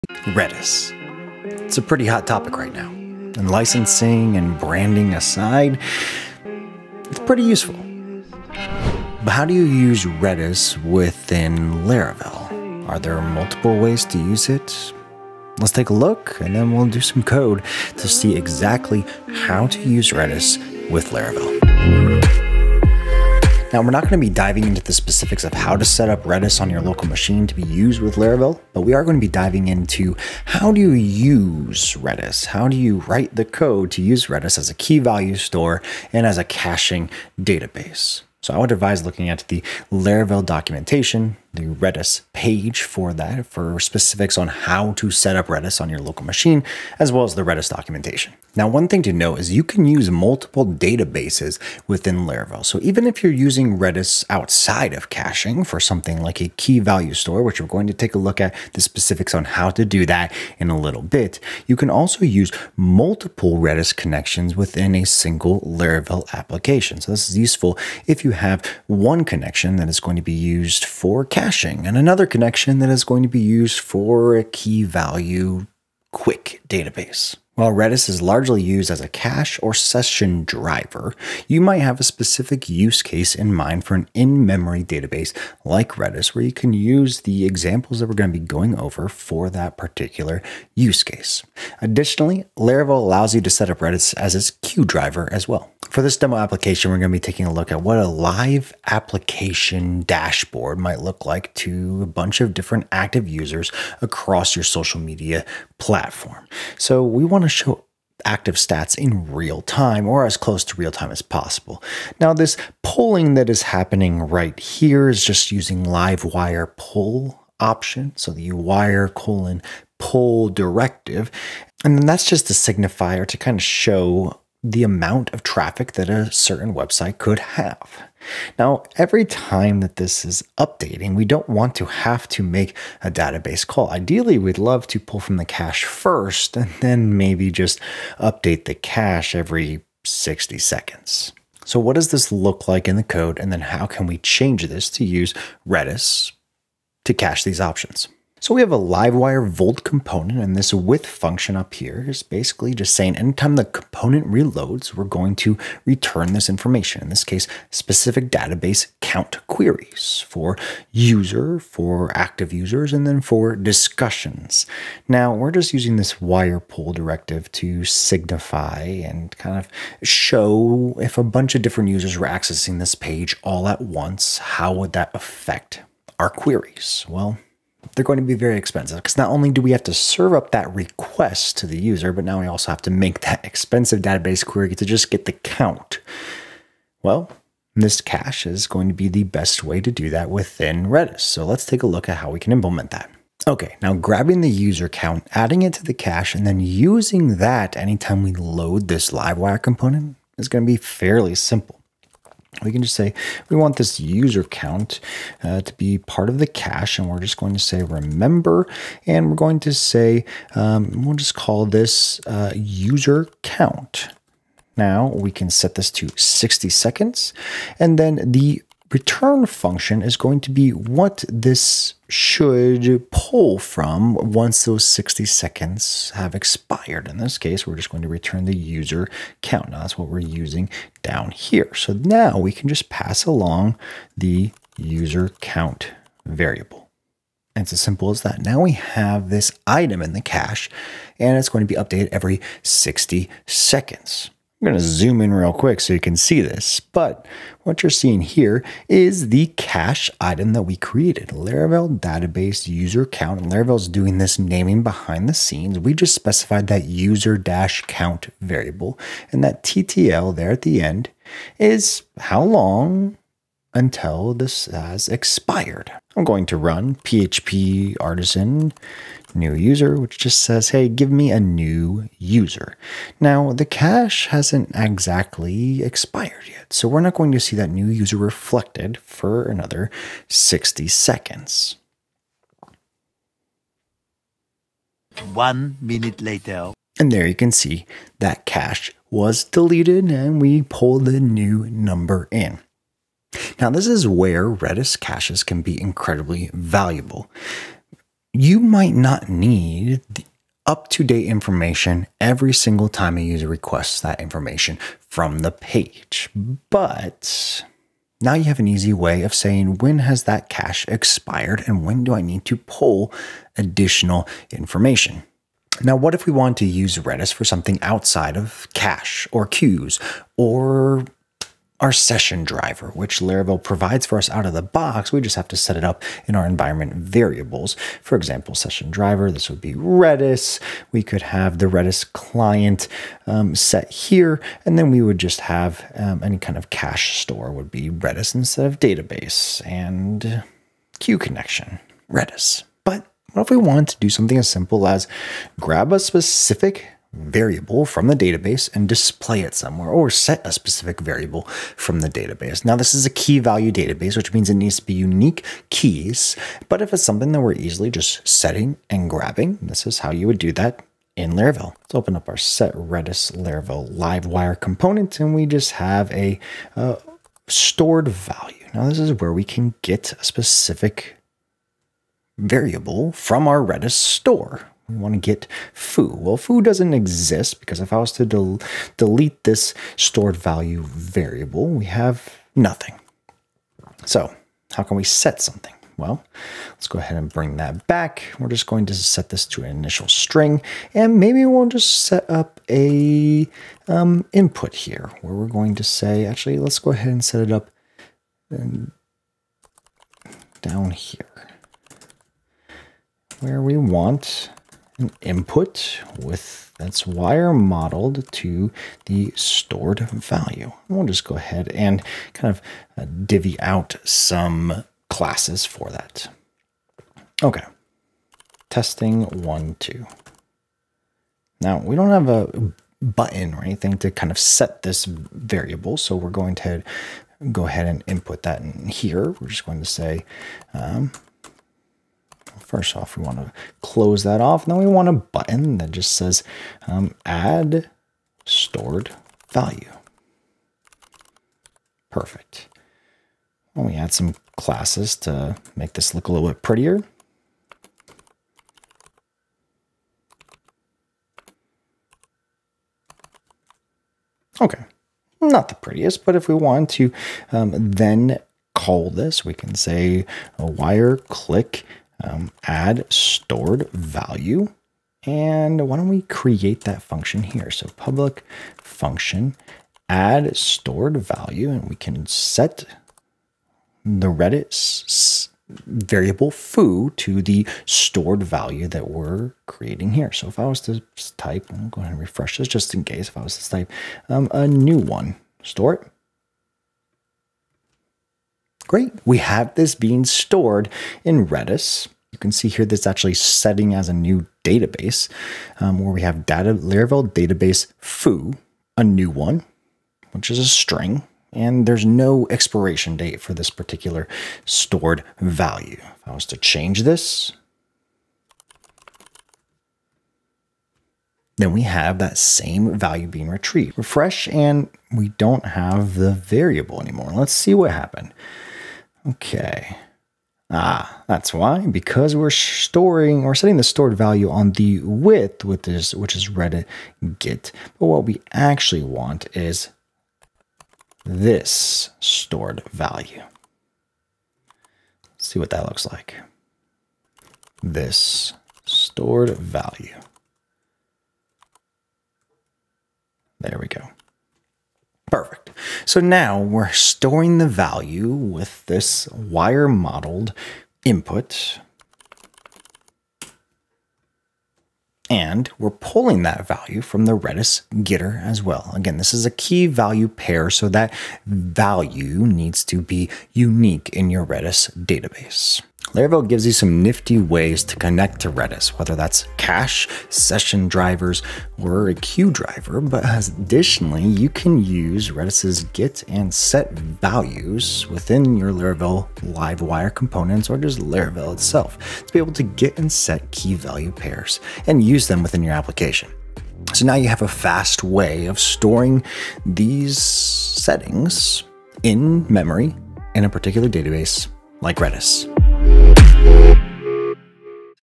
Laravel and Redis: The Absolute Basics
What does Redis even do? Apart from telling Laravel you want to use Redis as a session driver and just have magic happen behind the scenes, is there anything else that you can use Redis for within Laravel?

Let's take a look at the absolute basics and see if we get some ideas.

Keep creating.